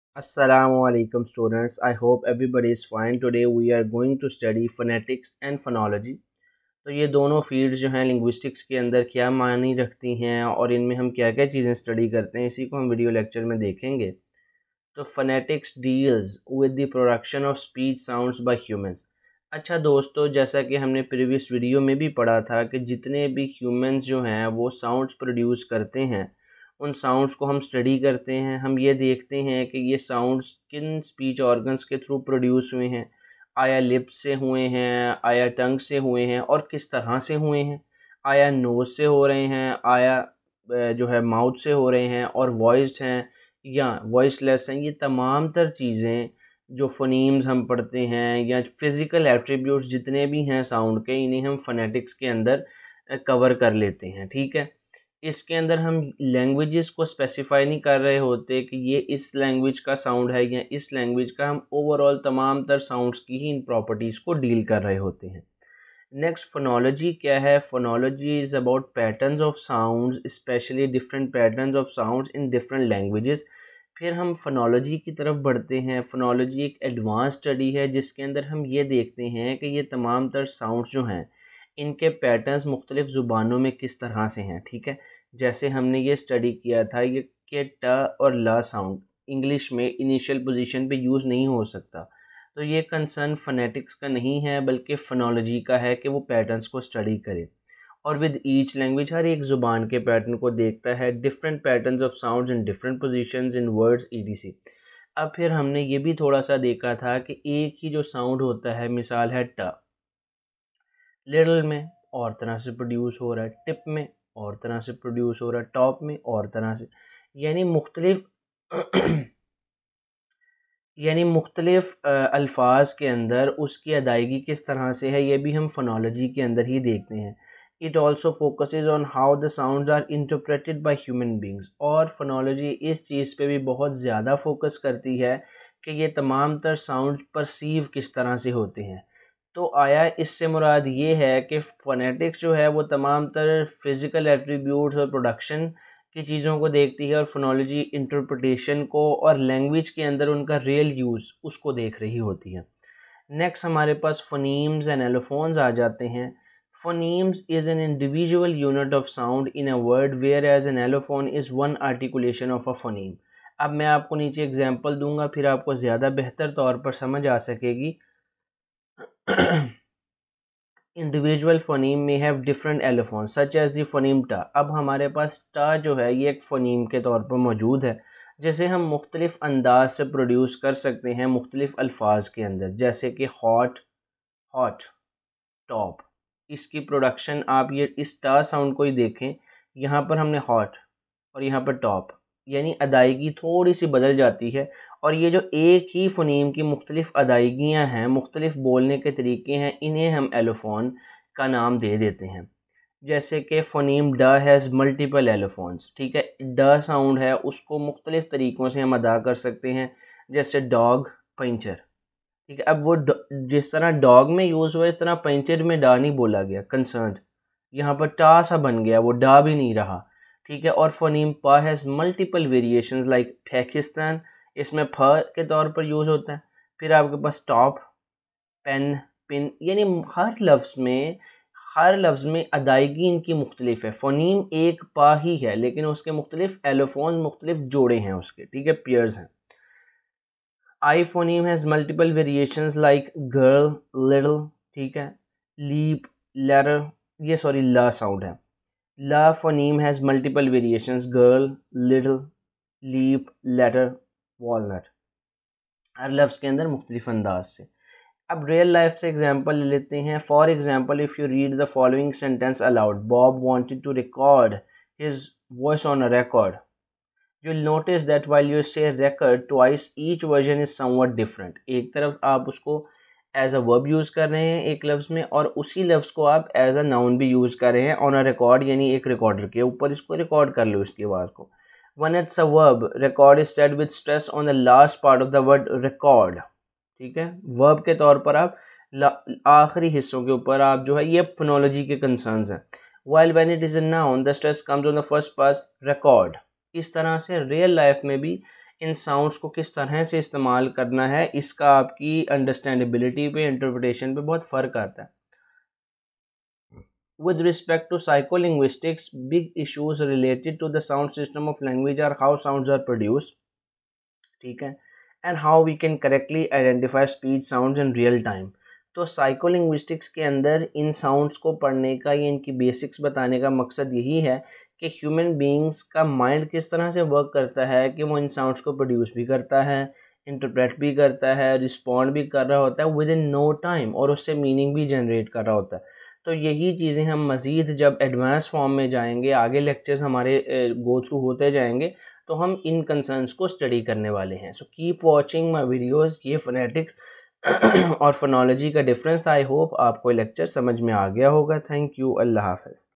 Phonetics & Phonology Linguistics | Phonemes vs Allophones | Real Life Examples
This video introduces the fields of Phonetics & Phonology, which study the sounds in human language.

TOPICS

Phonetics:
- Physical attributes of human sounds

Phonology:
- Phonemes
- Allophones
- Real-life examples